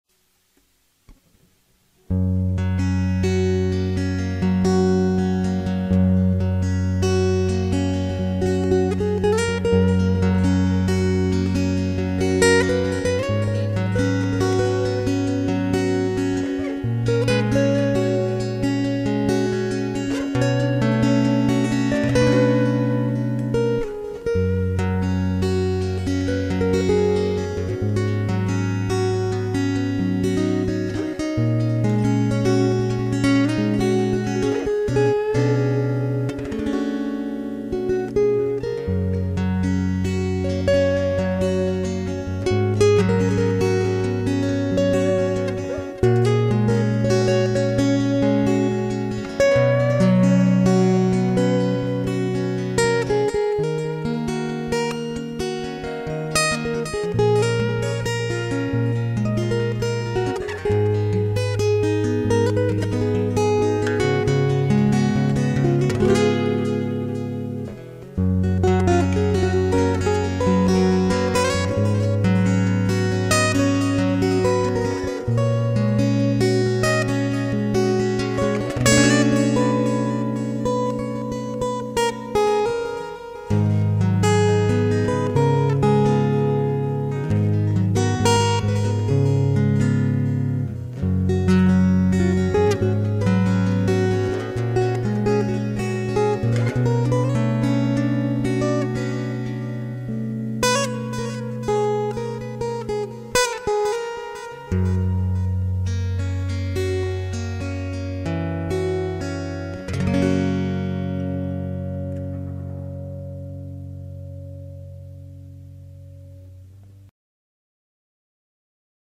K Yairi Kyf 3e Démo by JF Castronovo in 2017
uitare de fabrication artisanale par Kazuo Yairi (Japon)
- Guitare électro-acoustique demi-caisse
- Preampli à lampe: Fishman EQUIPPED
- Mecanique bain d’huile gotoh SG 381
- Table: Koa d’Hawai flammé
- Dos et éclisses: Acajou
- Manche: en acajou
- Touche : en ébène
- Chevalet : en ébène
- Largeur au sillet du haut: 42 mm
- Ouies: en f
- Contours de table : incrustations en abalone
- Forme du corps : Folk Orchestra/Pan coupé
- Date de fabrication: 1998
Remarque: l’action est basse